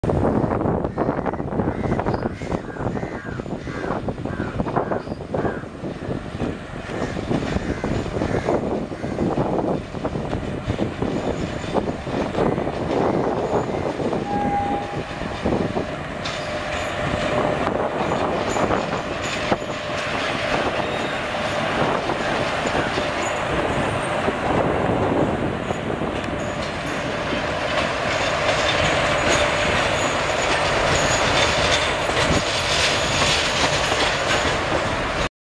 MEGA OFFLINK : BRAND NEW WAG-7 CHARMINAR EXPRESS
Train no : 12759, CHARMINAR EXPRESS
Loco : AJJ,WAG-7 28643
Location : Basin Bridge

12759 Charminar express covers 781 km in 13 h 5 min with 14 halts,average speed of 59 km/hr...

12760 HYB - MAS charminar express covers 781 km in 13 h 20 min with 14 halts,average speed of 58 km/hr